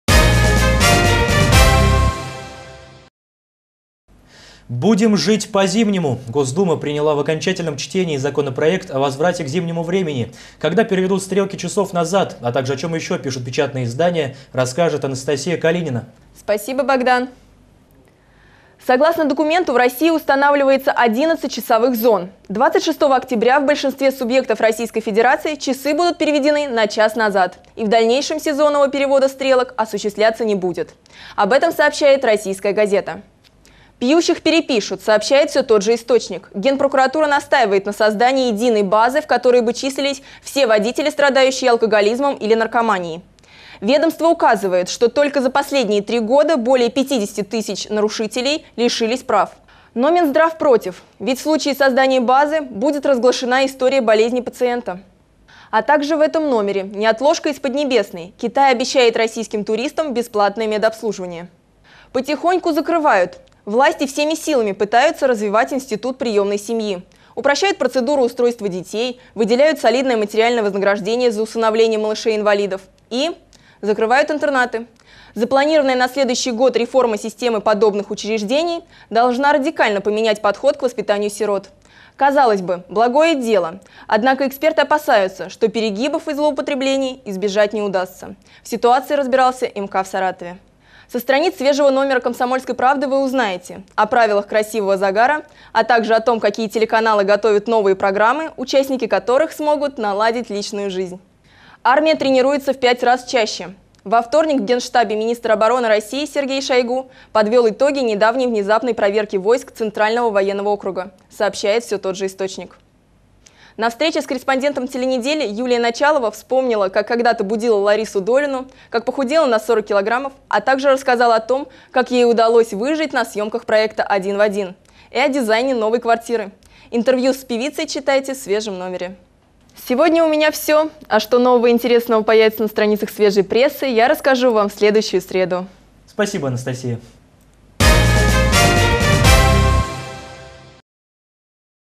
В России будут жить по зимнему времени
Госдума приняла в окончательном чтении законопроект о возврате к "зимнему времени". Когда переведут стрелки часов назад, а также, о чем еще пишут печатные издания, расскажет Анастасия Калинина. Сюжет вышел в эфир 2 июля.